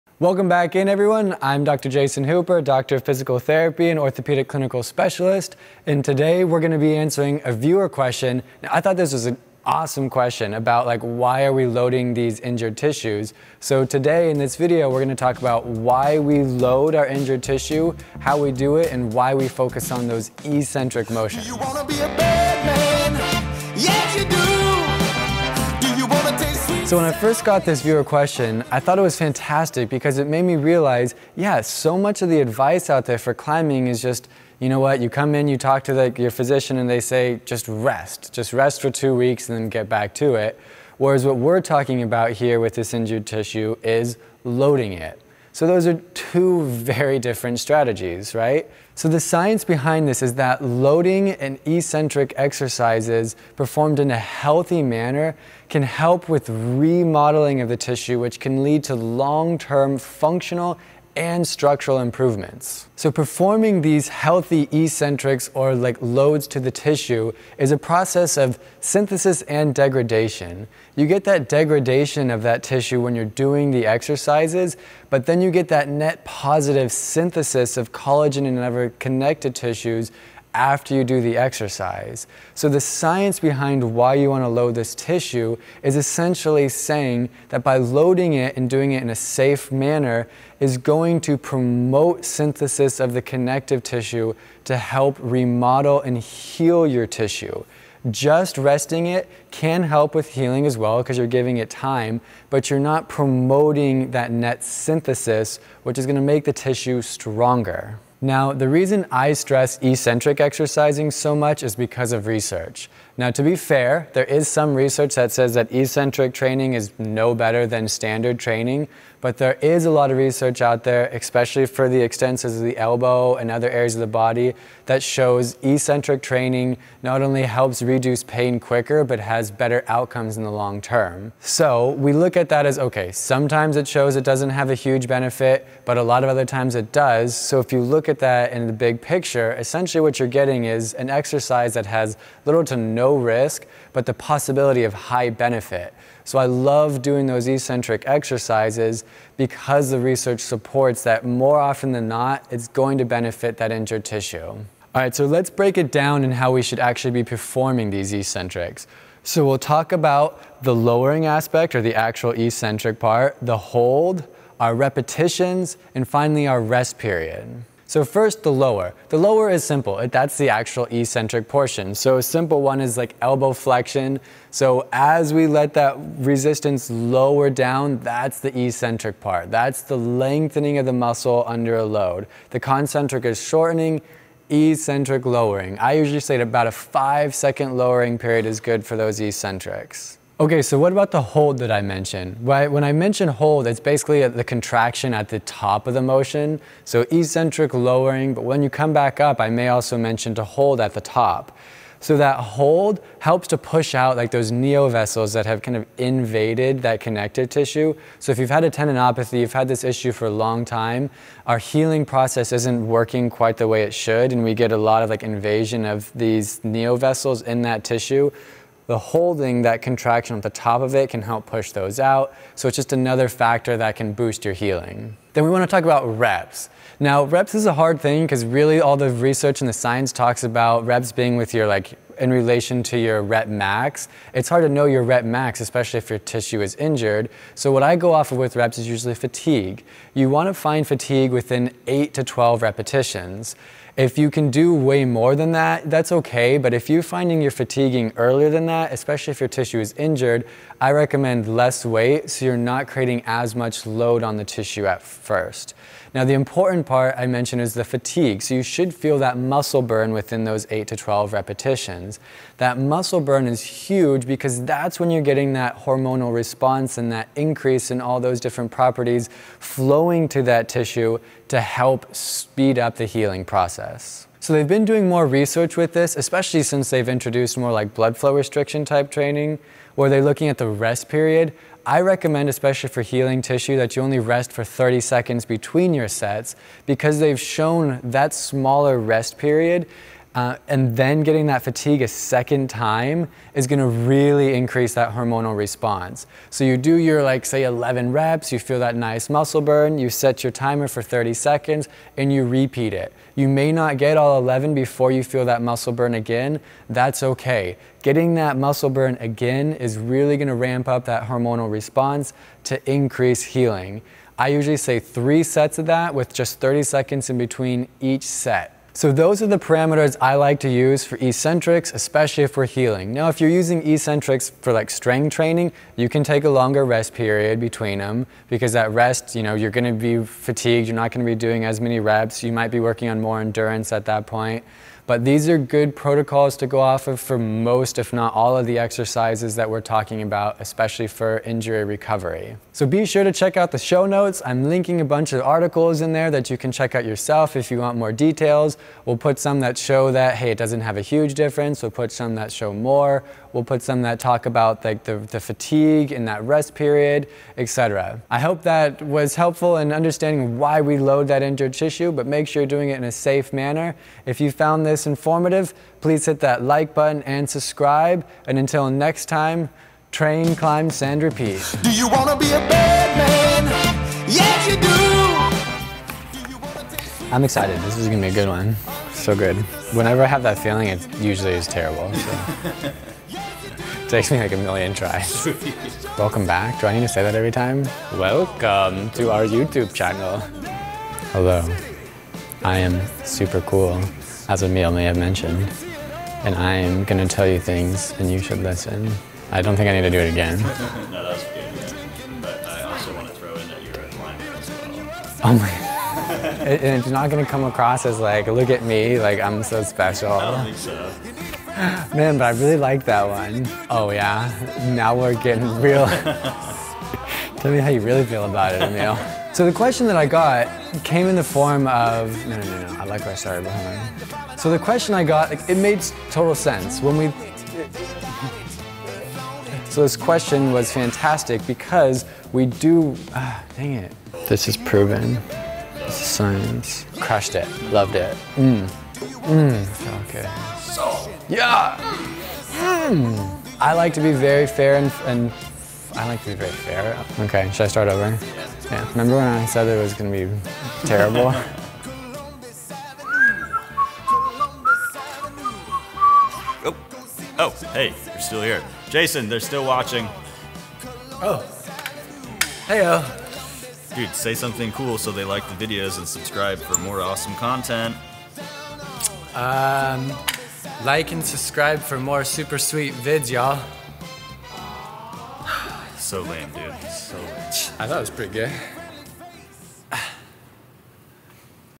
Why We Fix Climbing Injuries by Loading The Tissue Early
For more content like this as well as in-depth blog writeups and links to research articles, go
I recently got a great viewer question about why we want to load our injured tissue and why we do it with eccentrics. I think that's a fantastic question because if we don't know the research behind it, it seems counter-intuitive, especially when all of the advice we get is “rest”.

Intro (00:00)
Video Overview (00:26)
The Science Behind It (00:48)
Research (01:48)
Eccentric Guidlines (02:34)
Final Notes (05:19)
Outro & Bloopers (05:47)

So let's talk about why we load injured tissue and how we know how much load to do!

"Repetitive healthy loading, as in exercise, can promote remodeling in the tendon, leading to long-term structural and functional improvements. The process of tendon remodeling involves both synthesis and degradation of collagen with a net degradation that begins immediately after exercise and then shifts to a net synthesis." (Andarawis-Puri)

Essentially, what we are doing is loading the tissue in a SAFE manner, to create physiological demand on the tissue that forces the body to adapt to these changes. We want to increase the workload or work capacity of the muscle group. Unlike the activity we were doing to hurt the tissue, this type of loading is designed to help and remodel it.

Sometimes this is straight forward. If a muscle group is weakened or not well cross trained, it may not be able to handle the forces you are placing on it. For example, if you never train your wrist extensors, they may not have adapted to your climbing demands and cannot handle the loads you place on them with climbing whereas your flexor group might be perfectly capable of handling the same load. Basically, group A can tolerate this really hard activity while group B cannot, eventually this leads to group B burning out.

Sometimes it's tougher like with a wrist flexor issue. We already load that ALL the time when climbing, so why load it more? Well that's when we need to be even more specific and work on eccentrics to help with tissue remodelling.

Eccentrics
An eccentric contraction is the lengthening of a muscle under load. Concentric would be shortening under load. So why do I always stress eccentrics? Well, in rehab terms the evidence shows that eccentrics can help improve tendon remodeling which promote strong cellular structure of the tissue which improves strength and will speed up your healing time frame. While there is good research to support those, I would be biased if I didn’t mention that there is some research that shows it doesn't do more than regular strength training. So, on one hand we have evidence showing that it does help tissue, and on the other it shows that it is not more effective than regular strength training. So essentially we can view this as a high benefit low to no risk method of training. It is likely to help your healing while it is not likely to cause damage or be a waste of time.

The hold
I've also mentioned a hold before. The hold occurs at the end of a concentric contraction, so at the shortest length of a muscle and tendon. The reasoning behind this is to push out the neovessels that have inappropriately invaded the tissue. Often times when we have a chronic issue, we have disorganized healing and the invasion of said neovessels. This can actually slow the healing process so the hold functions to improve and restart the healing.

I often recommend only a 30 second rest between sets because there is research that shows an increase in release of certain hormones and chemicals with a shorter rest periods, in particular, 30 seconds of rest. If you are rehabbing injured tissue, this is clearly ideal. On the other hand, if you're training for strength or power you may want a longer rest, but we are looking at this as a healing method.

Repetitions (8-12, fatigue)
Now the hard part is repetitions. 8-12 is a great range to gauge the weight you're doing. You shouldn't have pain with reps in this range but you should feel muscle burn. If you're not feeling this then you won't be getting that good release of hormones and chemicals and you're not optimizing your healing. So if you are up to 16 reps and feeling no burn OR pain, increase the weight. If you have pain or a burn at 4 reps, drop the weight down.

Keep in mind, if you're doing the 30 second rest protocol, the reps may drop. You may start with 10, then only get 8 the second set and 6 the last. That's normal and acceptable, as long as you are stopping due to fatigue/burn and not due to pain. If you have pain and that's stopping the reps, lower the weight and listen to your body.

For the rest of the show notes including the references, please

Jason Hooper, PT, DPT, OCS, CAFS